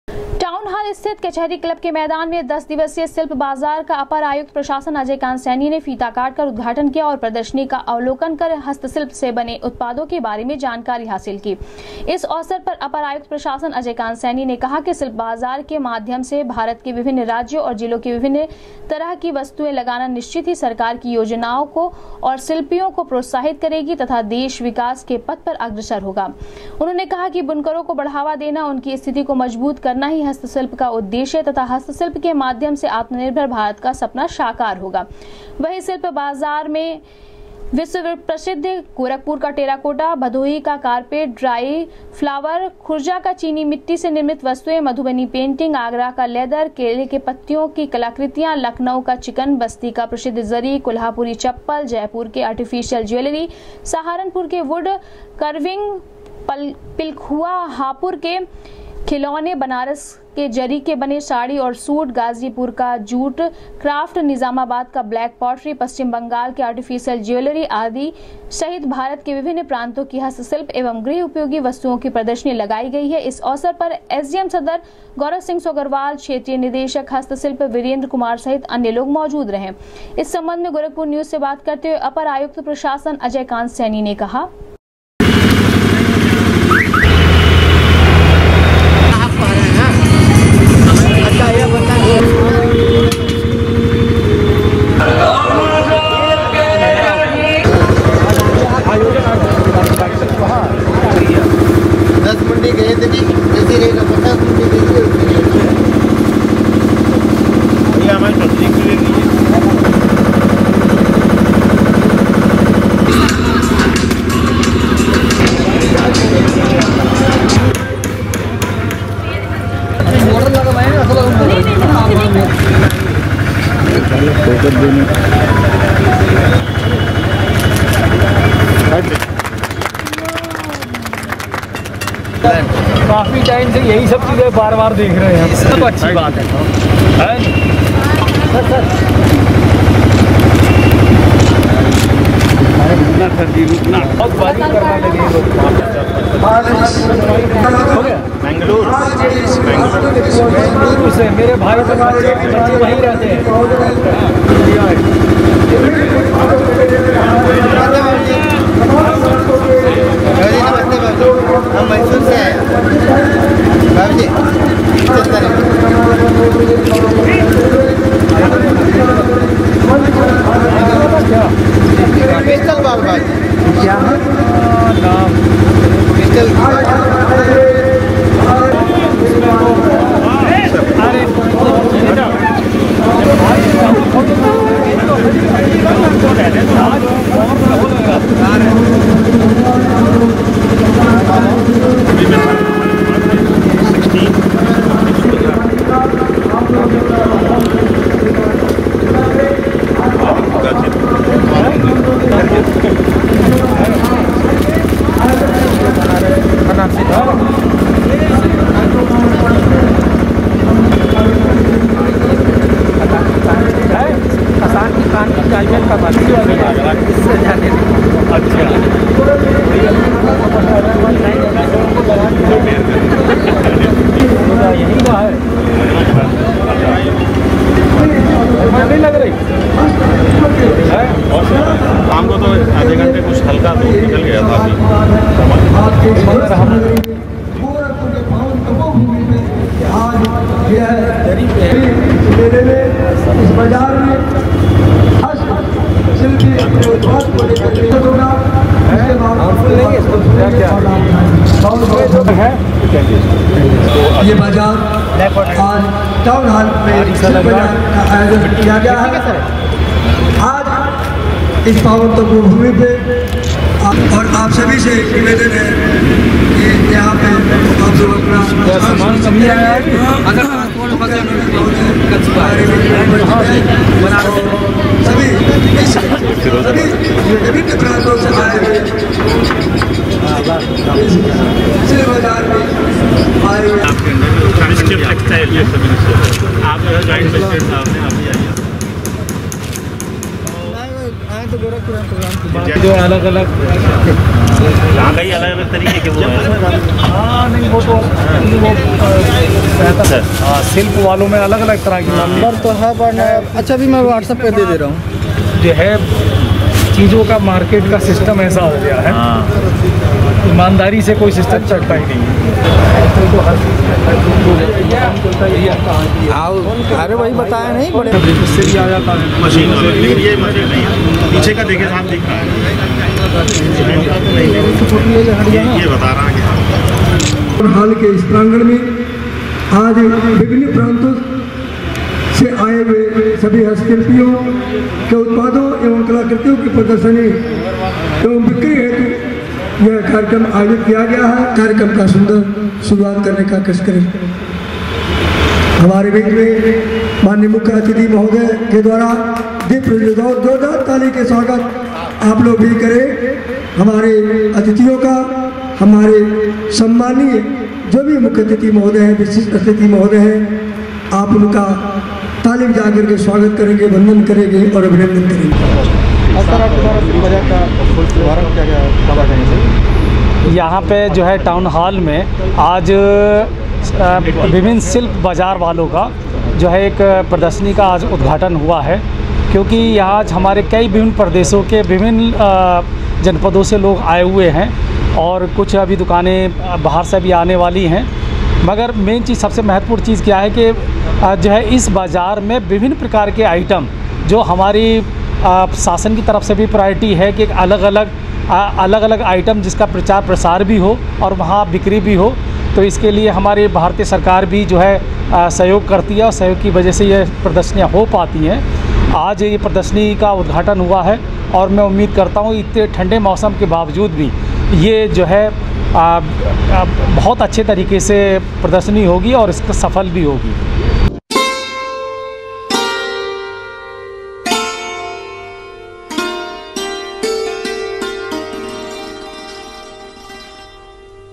10 दिवसीय शिल्प बाजार का अपर आयुक्त ने फीता काटकर किया उद्घाटन,कहा, प्रदर्शनी के माध्यम से हस्त
10 दिवसीय शिल्प बाजार का अपर आयुक्त ने फीता काटकर किया उद्घाटन,कहा, प्रदर्शनी के माध्यम से हस्त शिल्प को मिलेगा बढ़ावा,आत्मनिर्भर बनगे शिल्पकार